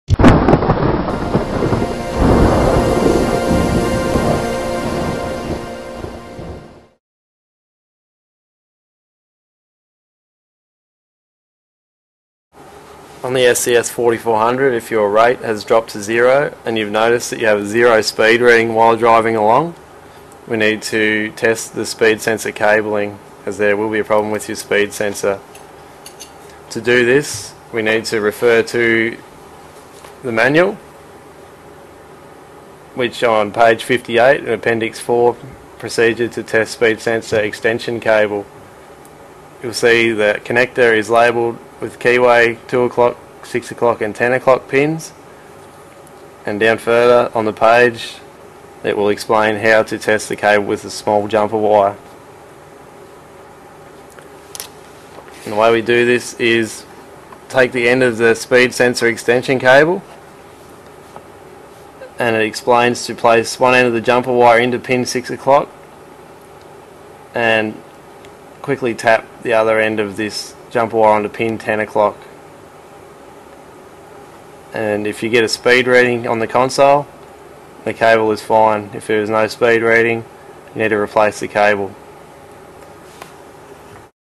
GoldAcres Raven component testing
Testing the speed sensor cabling